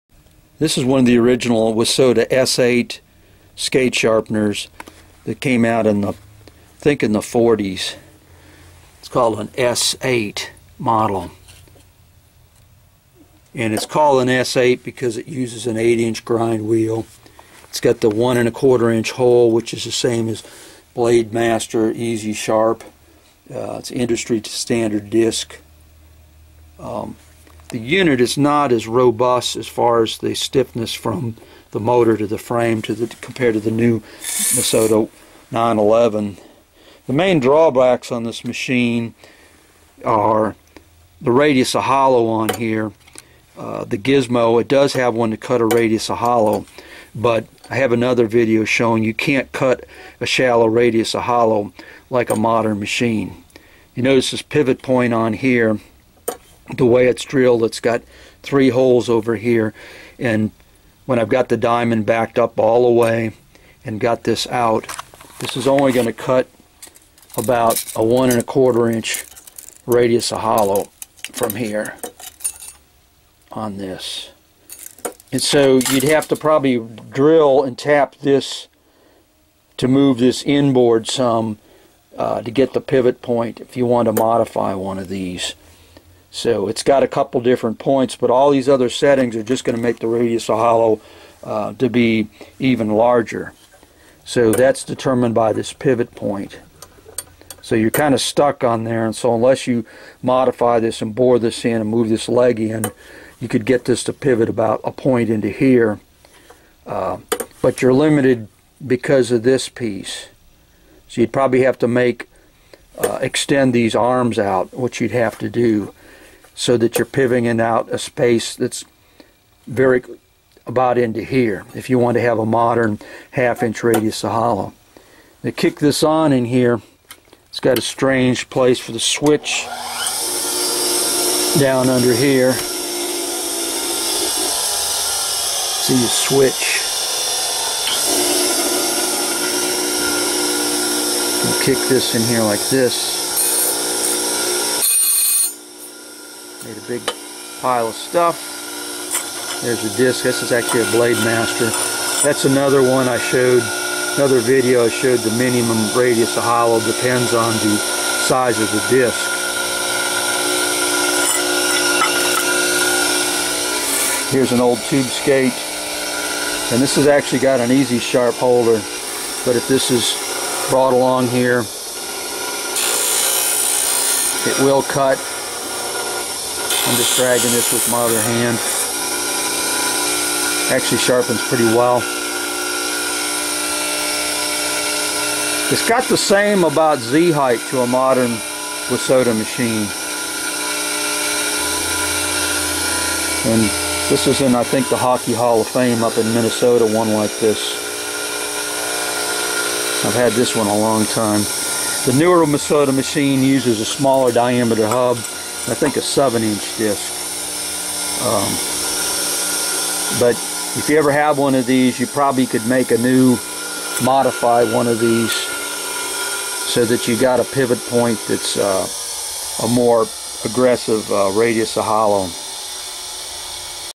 Wissota S8 Skate sharpener
These units were made from the 1940s to say maybe 1980s?. Unit uses a 1 1/4 inch hub 8 inch disk, unlike the modern Wissota 911 model. Limitations for this model are radius of hollow cannot be cut as shallow as a modern machine. See my other video on this.The motor spindle to work"bed" is less stiff than a stiff wissota 911of today.

 3beltwesty 8k sny2